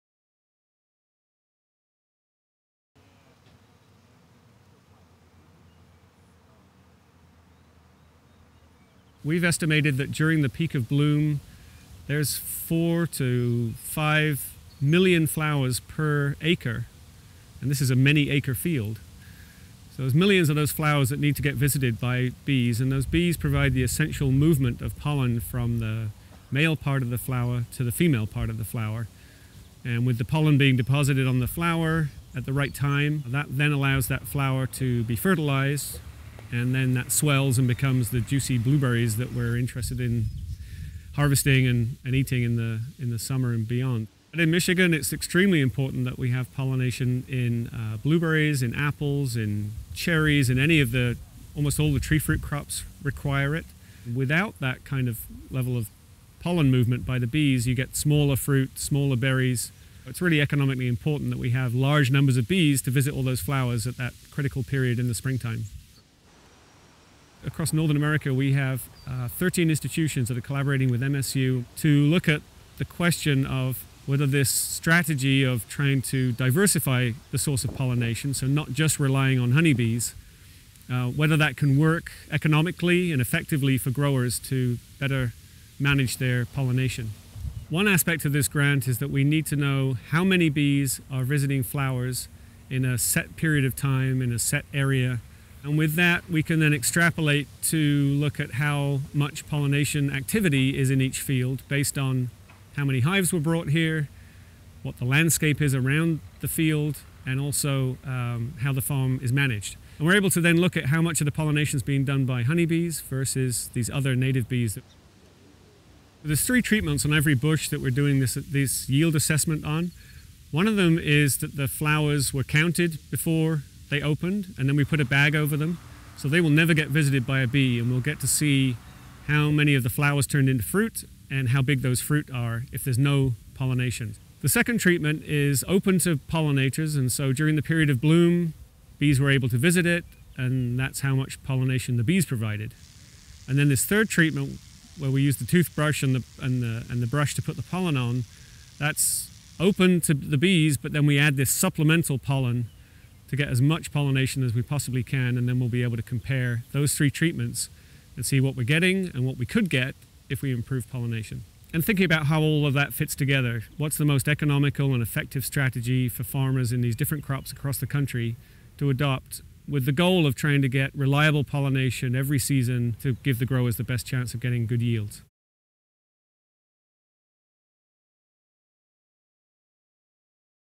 Going wild could improve winged workforce
Every spring in the United States, bees pollinate crops valued at about $14 billion.

A Michigan State University professor and a team of scientists are using a five-year, $8.6 million grant from the United States Department of Agriculture to keep this winged workforce operating efficiently.

Almonds, strawberries, apples, cherries, blueberries, raspberries, watermelon, cucumbers and more depend on bees to help maximize yields. But with wild honey bee populations decimated by varroa mites and other threats, farmers are dependent on beekeepers to deliver managed colonies of honey bees during peak pollination to ensure their flowers are pollinated.